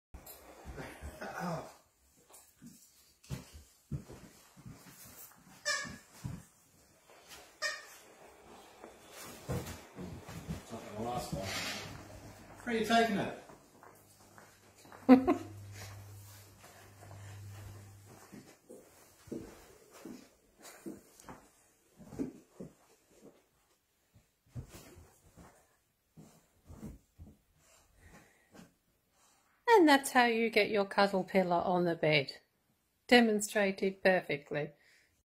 Try not to Laugh | Greyhound vs Huge Toy | Funny video | Full HD | Don't die Laughing |Funny Animals
Hello Everyone,
Greyhound vs Cuddle Pillar
When your human buys you a giant toy and you just have to take it everywhere with you.